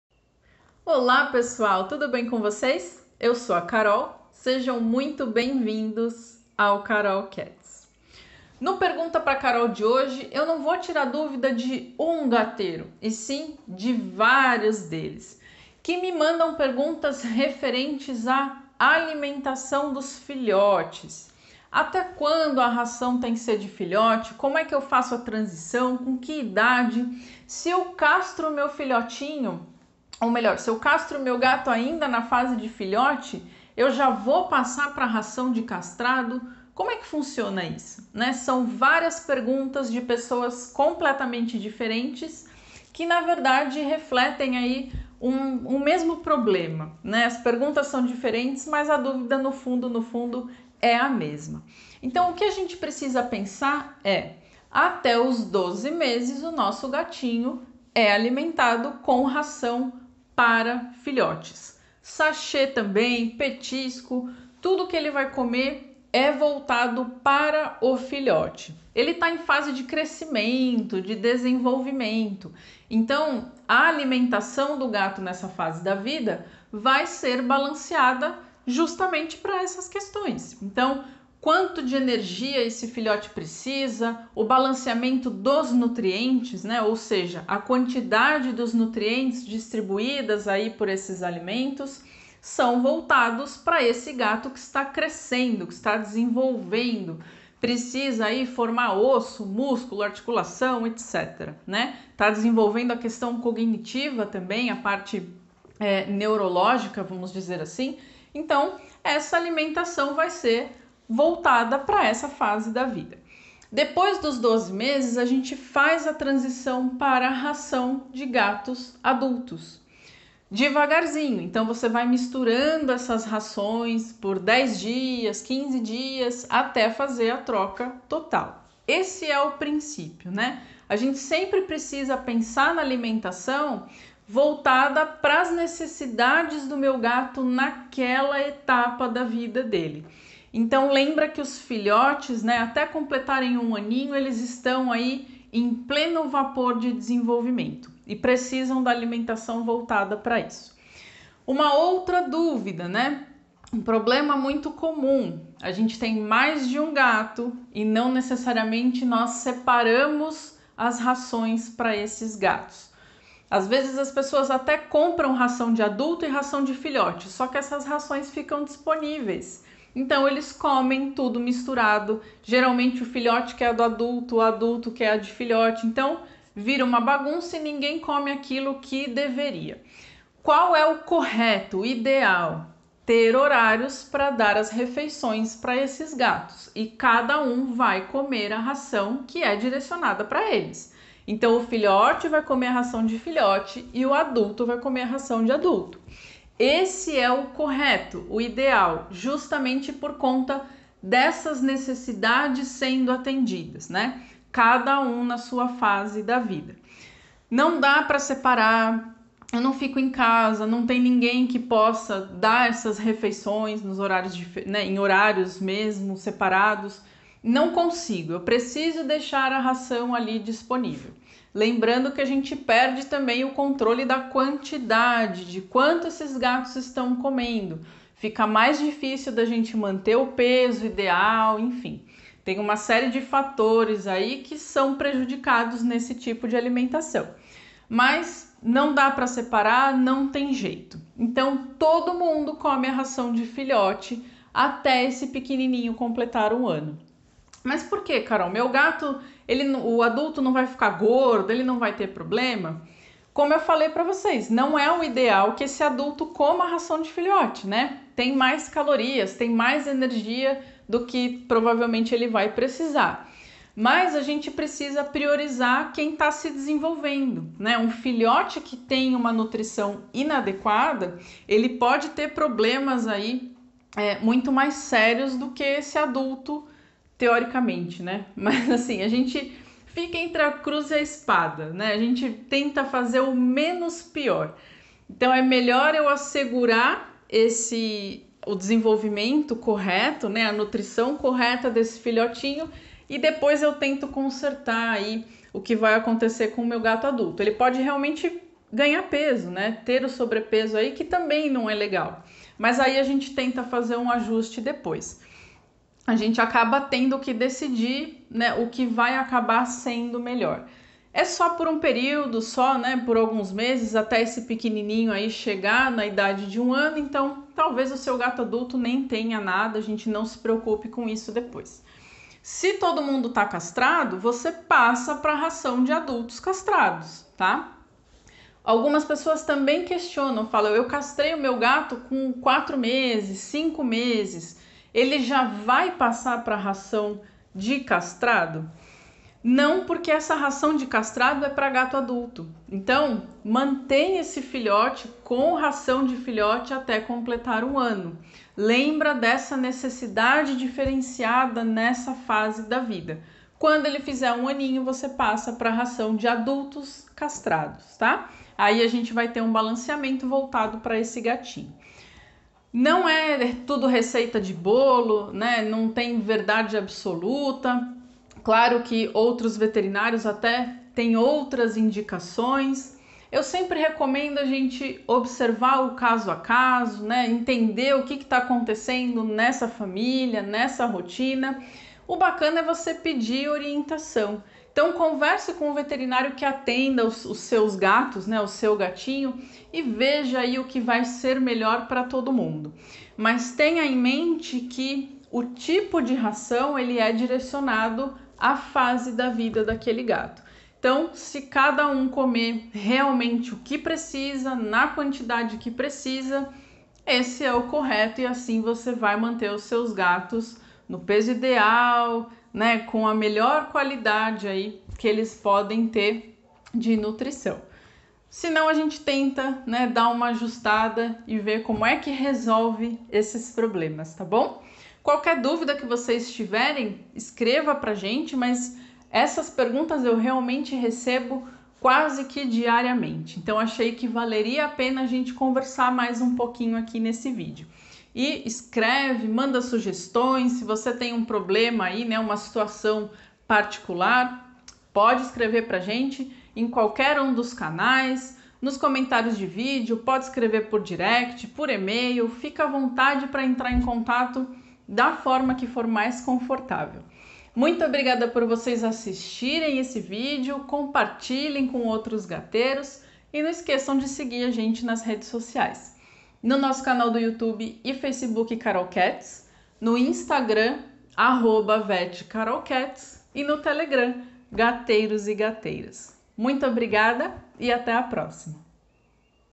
DÚVIDAS SOBRE RAÇÃO DE FILHOTES
Olá gateiros e gateiras,

Diariamente eu recebo dúvidas com relação a alimentação dos filhotes. Qual a ração indicada, quando trocar, pode comer ração de adultos, tenho mais de um gato. e agora?! Nesse vídeo eu esclareço algumas dessas questões para te ajudar a oferecer a melhor nutrição para o seu pequeno(a).

Você já viveu algum dilema desses? Então conta pra gente!